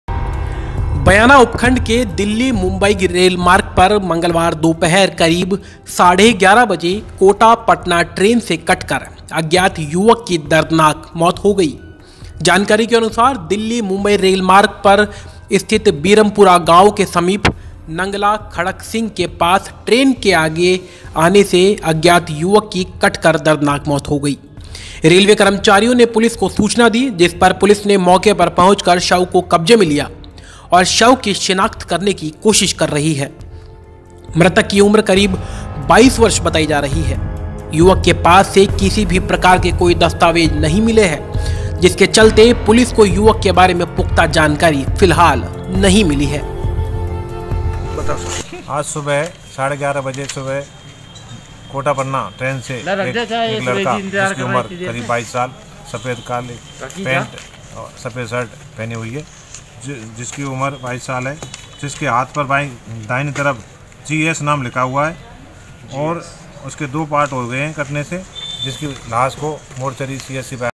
बयाना : ट्रेन से कटकर युवक की दर्दनाक मौत
दस्तावेजों के आभाव में नहीं हो पायी पहचान